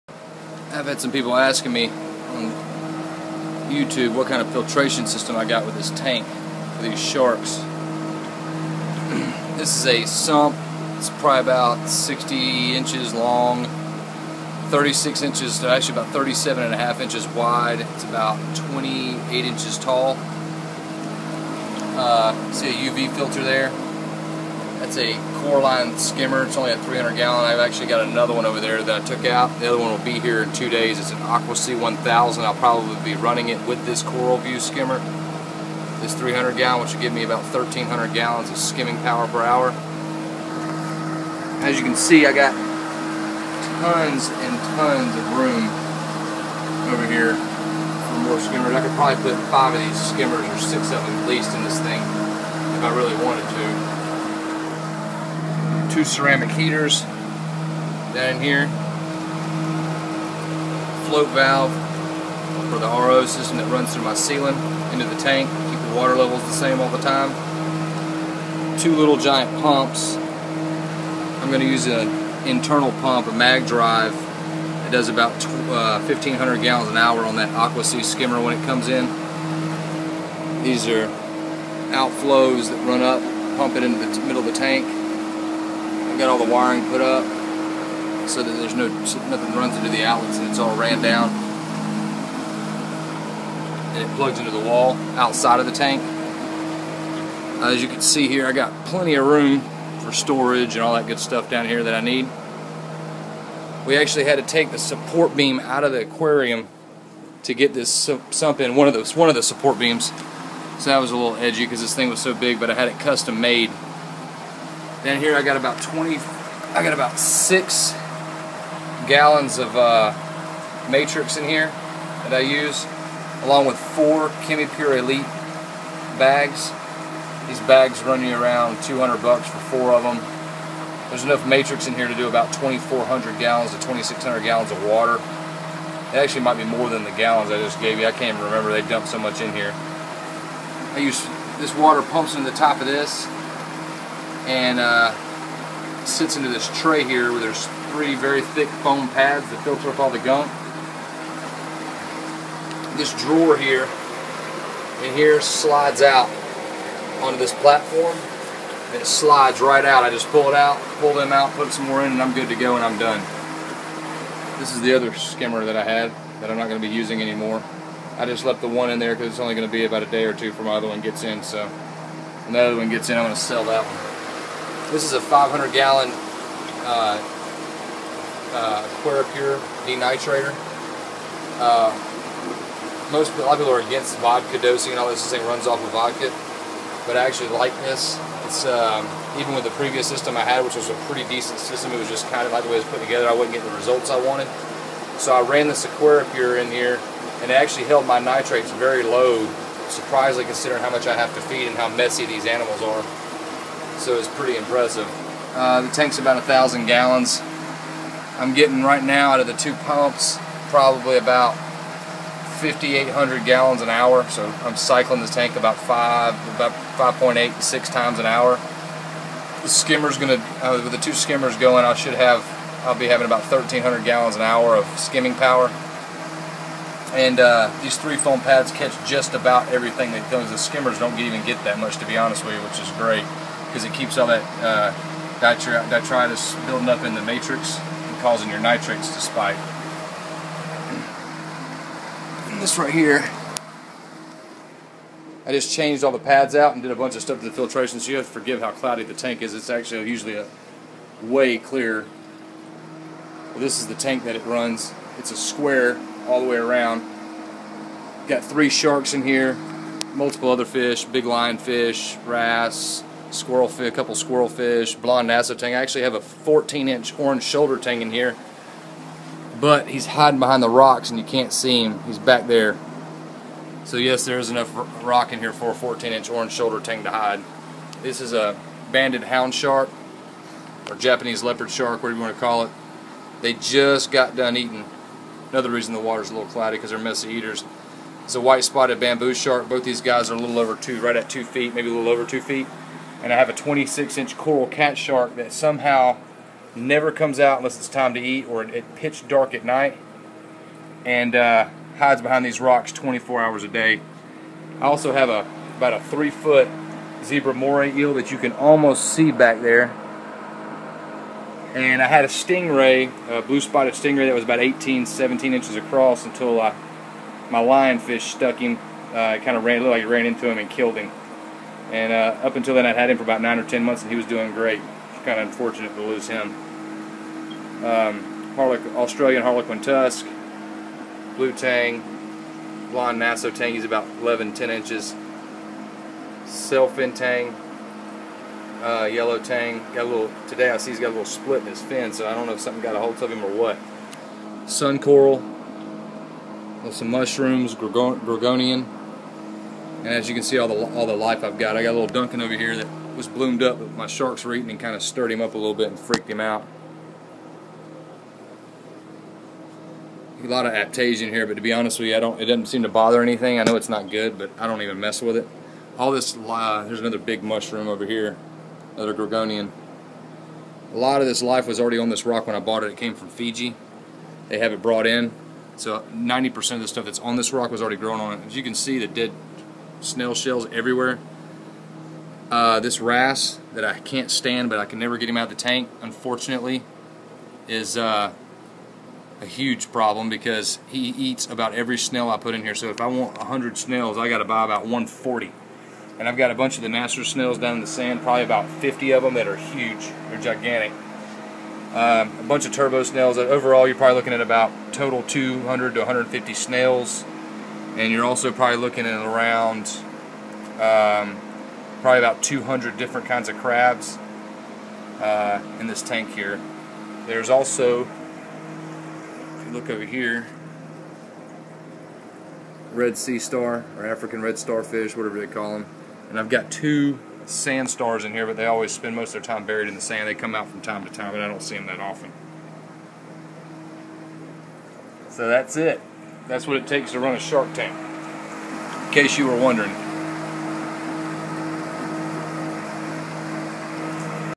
Shark aquarium Set Up. Everything you need on a home shark tank.
I had  quite a few people asking and emailing me what it takes for a shark set up.  So here you go.    I go into pretty good detail regarding everything.   Sharks I have found are far easier to keep then most fish I have had.   As long as you spend the money and buy the proper equipment along with the space.   This tank turns about 6,000 gallons per hour or close to it and when the new skimmer comes in tomorrow will skim 1,300 gallons per hour.   The sump is a little over 5ft long  and a little over 3ft wide.  It was custom made.   If you want sharks do not go cheap on space and equipment and you will be just fine.

Good Rules to follow
Best small shark for smaller tank - Coral Catshark gets 27 inches and can live in a 5ft by 2ft tank.

Great Book to read - Sharks and Rays.    Best book around on sharks as pets

When all else fails add more filtration.  No shark has ever died because his water is to clean.

Use Vitamins on the food.  Spend the money on the little things like this and you will be problem free.